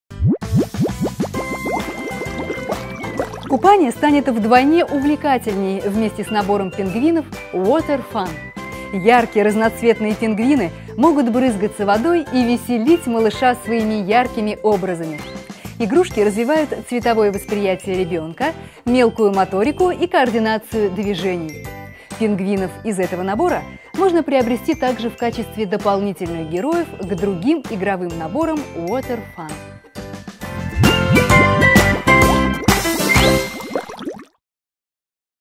WATER FUN "Веселые пингвины" игрушки для воды
Water Fun "Веселые пингвины" это игрушка для ванны. Набор из трех мягких пингвинчиков, которыми можно весело брызгать водой. Снизу к них присоски, что позволяет их закрепить на гладкой поверхности ванной или кафеле.